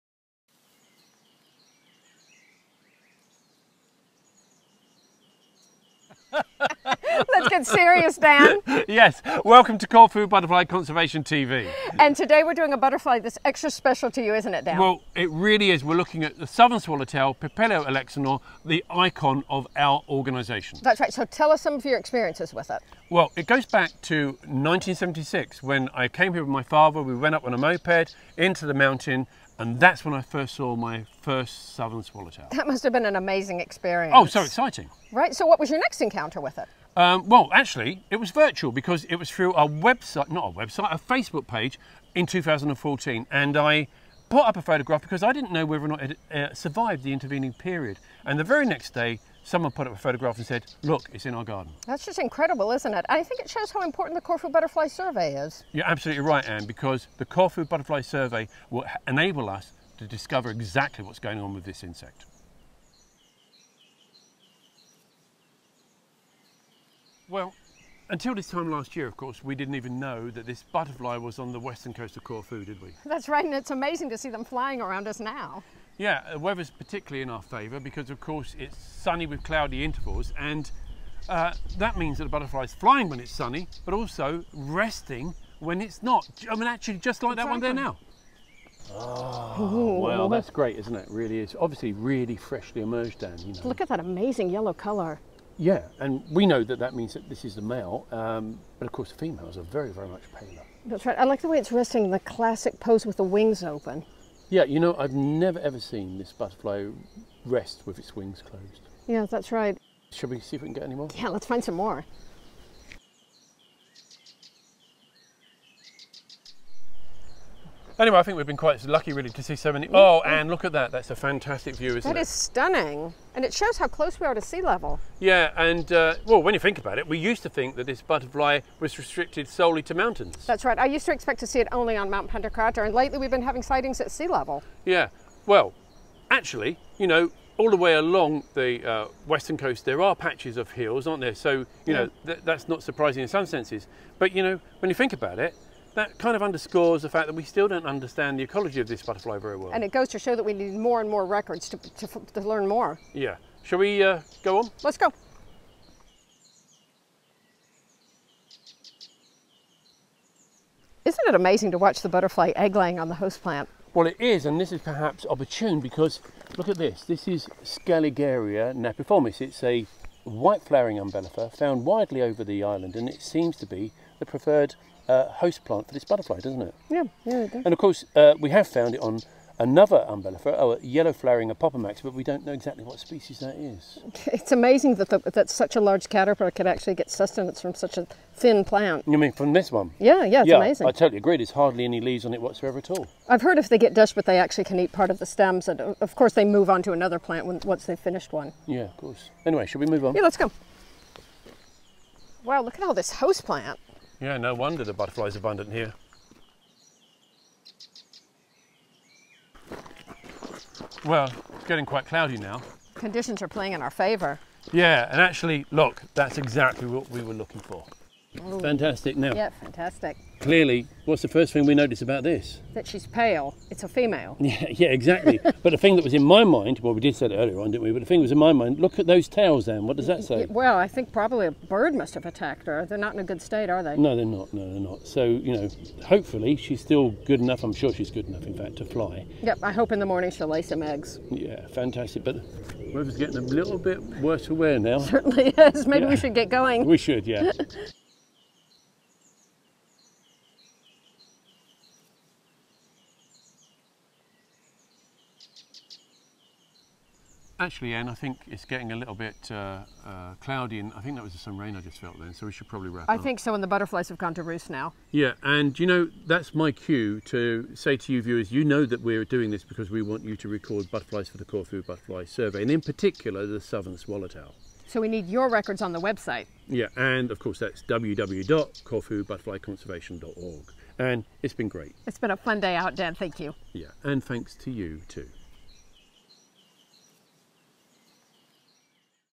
The Southern Swallowtail (Papilio alexanor)
The Southern Swallowtail - Papilio alexanor is now on the wing during May, in Corfu. We really need your records because we think this species might be restricted to regions with underlying limestone but we cannot be sure about this without records from all over the island.

Thanks go to Gillian & Ken Elsom, Emma Whitfield and Graham Durrant for their help in the making of this film.